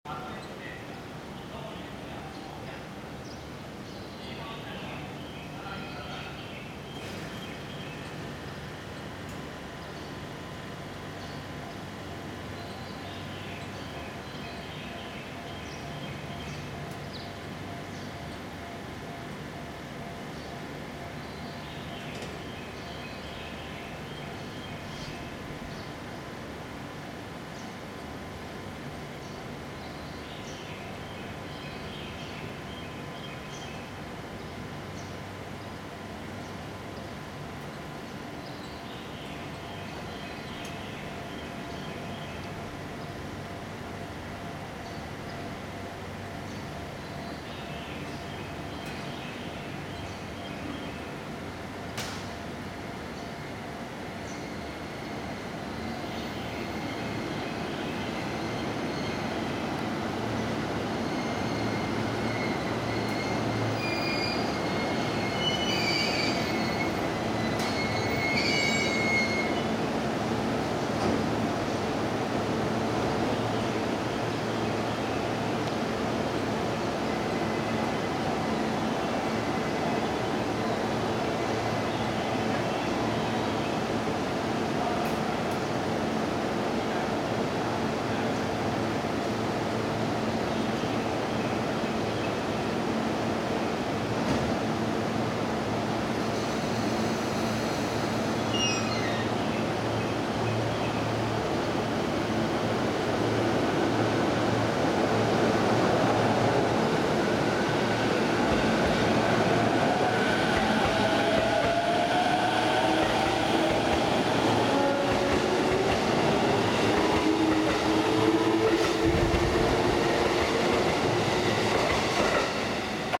Ｅ２３３系６０００番台横浜線根岸線直通各停磯子行き相模原駅入線～発車その１
クラＨ０２５編成、２０１４．８．３撮影です。